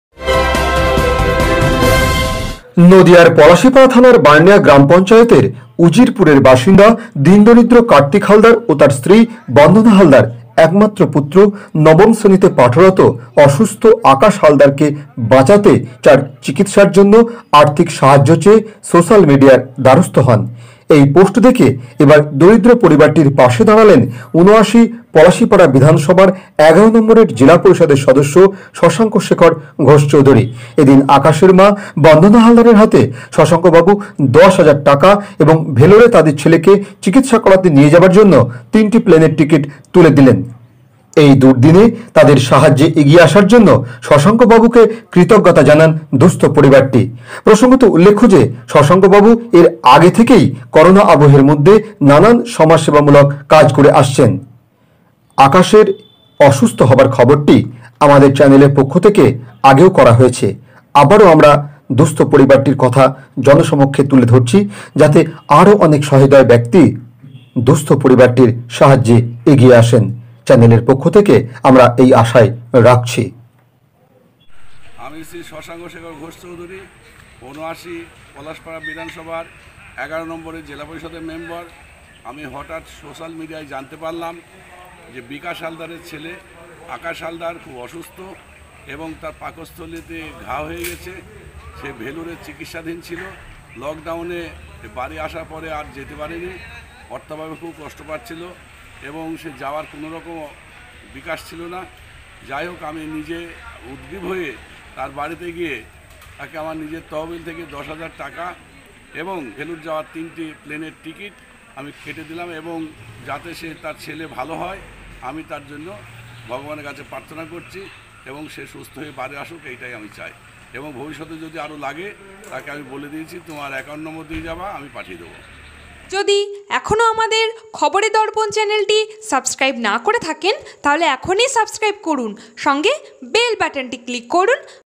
অসুস্থ ছাত্রের চিকিৎসার দায়িত্ব নিলেন জেলা পরিষদ সদস্য শশাঙ্ক শেখর ঘোষ চৌধুরী
সোশ্যাল মিডিয়ার খবরের জেরে অসুস্থ ছাত্রের চিকিৎসার সাহায্যে এগিয়ে এলেন জেলা পরিষদ সদস্য শশাঙ্ক শেখর ঘোষ চৌধুরী।।